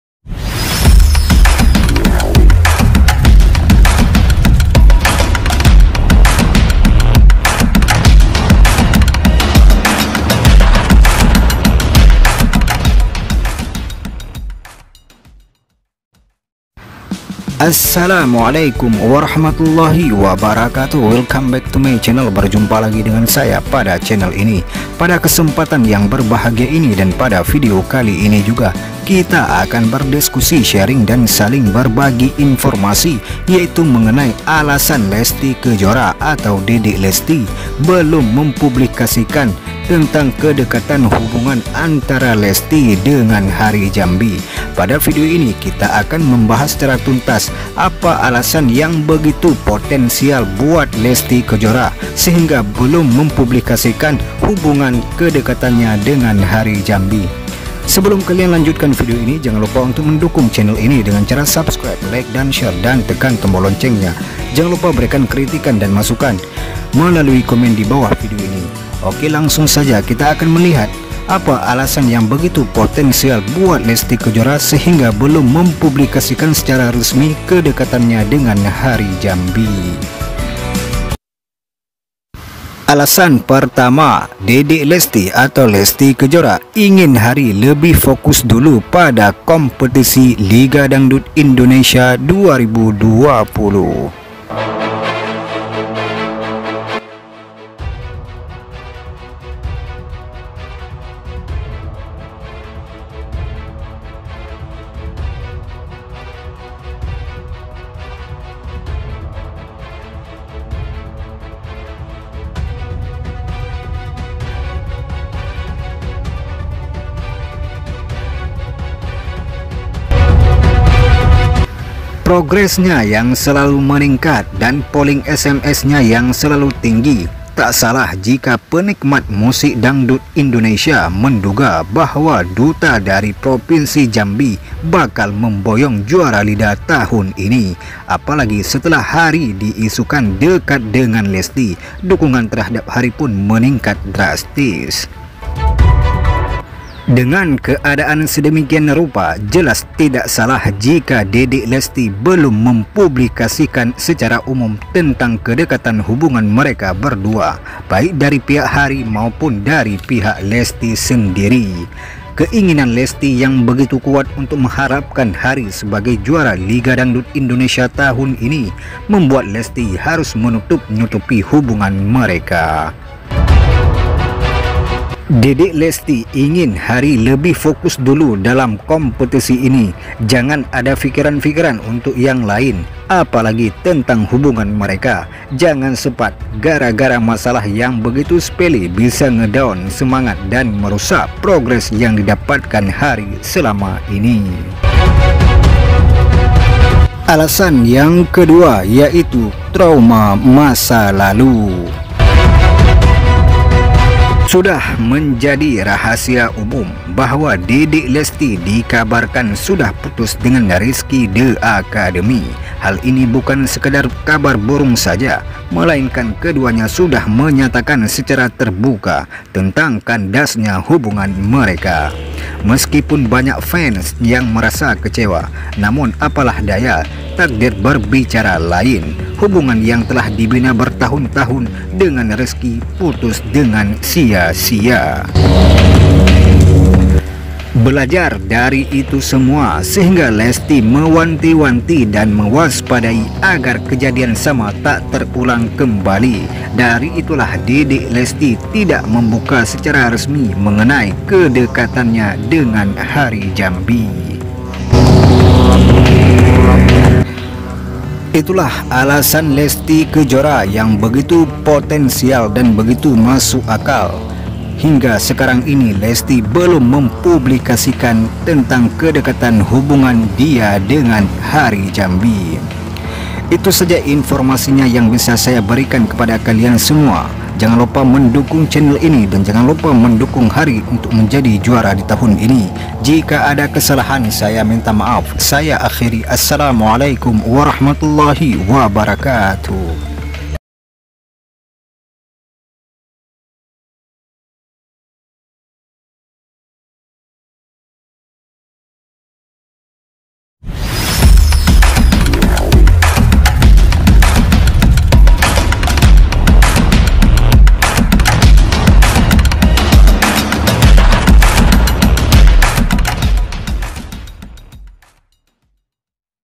LESTY MAKIN SAYANG PADA HARI HINGGA LAKUIN INI SEBAGAI BUKTI CINTA || HARI LESTY LIDA INDOSIAR
DEMI HARI JAMBI DEDEK LESTY RELA LAKUKAN INI KEPADANYA || HARI LESTY LIDA INDOSIAR

HUBUNGAN ASMARA ANTARA LESTY DAN HARI JAMBI SEMAKIN SERIUS. SAMPAI-SAMPAI DEDEK LESTY ATAU LESTY KEJORA RELA LAKUKAN INI KEPADANYA. LESTY MERUPAKAN SALAH SATU DEWAN JURI DI AJANG LIGA DANGDUT INDONESIA 2020 ATAU LIDA INDOSIAR 2020 SEDANGKAN HARI MERUPAKAN SALAH SATU PESERTA LIDA INDOSIAR 2020 ATAU LIDA 2020.

TERIMA KASIH SUDAH MENGUNJUNGI CHANNEL INI DAN SUDAH MENONTON VIDEO INI.